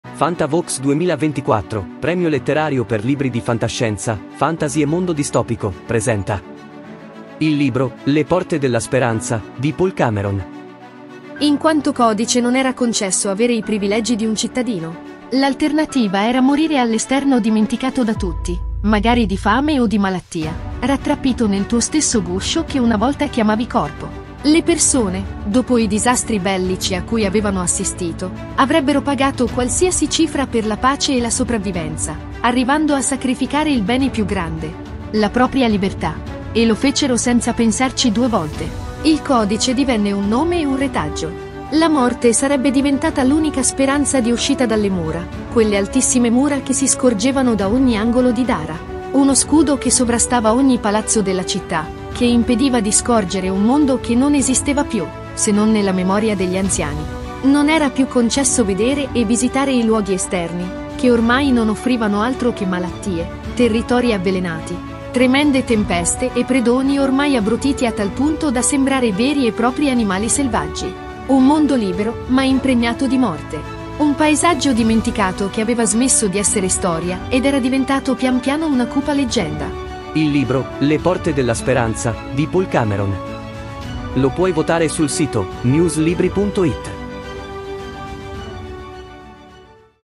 Le porte della speranza di Paul Cameron - FANTAVOX 2024
In quanto Codice non era concesso avere i privilegi di un Cittadino. L'alternativa era morire all'esterno dimenticato da tutti, magari di fame o di malattia, rattrappito nel tuo stesso guscio che una volta chiamavi corpo. Le persone, dopo i disastri bellici a cui avevano assistito, avrebbero pagato qualsiasi cifra per la pace e la sopravvivenza, arrivando a sacrificare il bene più grande: la propria libertà. E lo fecero senza pensarci due volte.
Il codice divenne un nome e un retaggio. La morte sarebbe diventata l'unica speranza di uscita dalle mura, quelle altissime mura che si scorgevano da ogni angolo di Dara. Uno scudo che sovrastava ogni palazzo della città, che impediva di scorgere un mondo che non esisteva più, se non nella memoria degli anziani. Non era più concesso vedere e visitare i luoghi esterni, che ormai non offrivano altro che malattie, territori avvelenati, tremende tempeste e predoni ormai abbrutiti a tal punto da sembrare veri e propri animali selvaggi. Un mondo libero, ma impregnato di morte. Un paesaggio dimenticato che aveva smesso di essere storia ed era diventato pian piano una cupa leggenda.

newslibri.it
noi promuoviamo libri